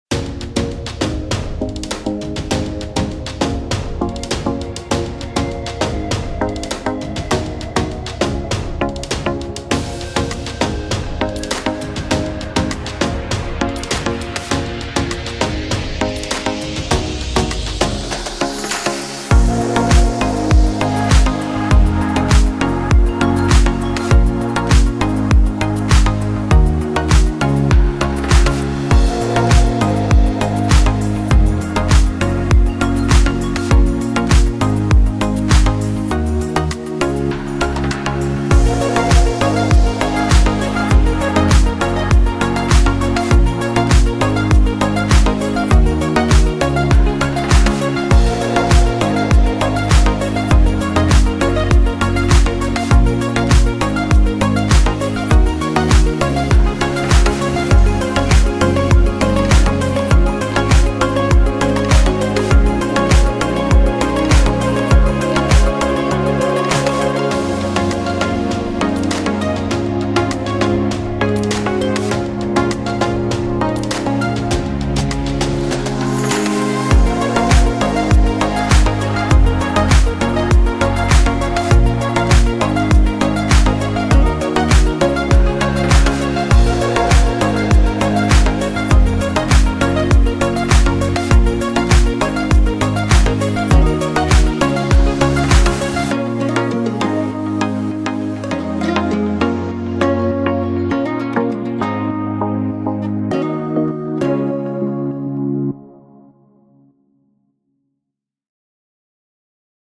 Claypaky Highlights @ Prolight+Sound 2022
Last week was something extraordinary: back on the trade show floor at Prolight+Sound in Frankfurt, with visitors enjoying the show and meeting each other again in person! Here some highlights from our booth!